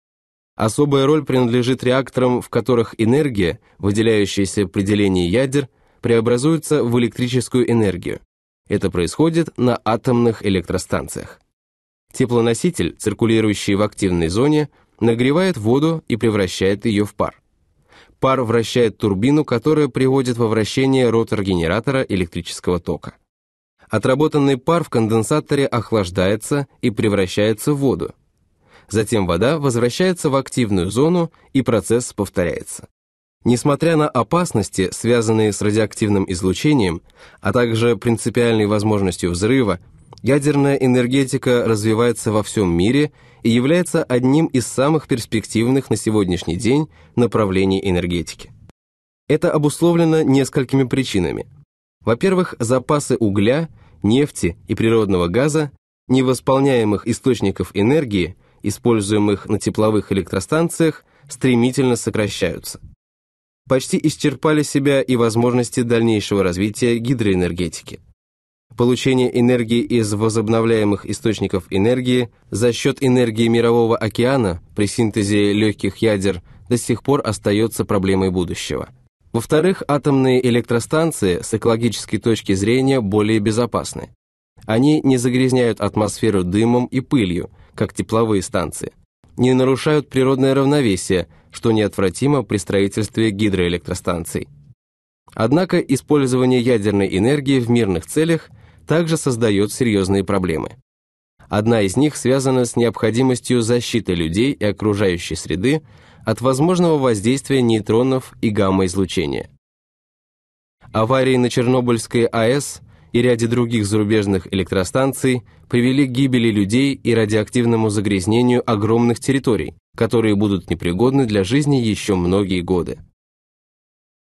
Атомные электростанции
Особая роль принадлежит реактором, в которых энергия, выделяющаяся при делении ядер, преобразуется в электрическую энергию.
Это происходит на атомных электростанциях.
Теплоноситель, циркулирующие в активной зоне, нагревает воду и превращает ее в пар.
Пар вращает турбину, которая приводит во вращение ротор генератора электрического тока.
Отработанный пар в конденсаторе охлаждается и превращается в воду.
Затем вода возвращается в активную зону и процесс повторяется.
Несмотря на опасности, связанные с радиоактивным излучением, а также принципиальной возможностью взрыва, ядерная энергетика развивается во всем мире и является одним из самых перспективных на сегодняшний день направлении энергетики.
Это обусловлено несколькими причинами: во-первых запасы угля, нефти и природного газа, не восполняем их источников энергии, используемых на тепловых электростанциях, стремительно сокращаются.
Почти исчерпали себя и возможности дальнейшего развития гидроэнергетики.
Получение энергии из возобновляемых источников энергии, за счет энергии мирового океана, при синтезе и легких ядер, до сих пор остается проблемой будущего; во вторых атомные электростанции, с экологической точки зрения, более безопасны.
Они не загрязняют атмосферу дымом и пылью, как тепловые станции, не нарушают природное равновесие, что неотвратимо при строительстве гидроэлектростанций.
Однако использовании ядерной энергии в мирных целях также создает серьезные проблемы.
Одна из них связана с необходимостью защиты людей и окружающей среды от возможного воздействия нейтронов и гамма-излучения.
Аварии на Чернобыльской АЭС и ряде других зарубежных электростанций привели гибели людей и радиоактивному загрязнению огромных территорий, которые будут непригодны для жизни еще многие годы.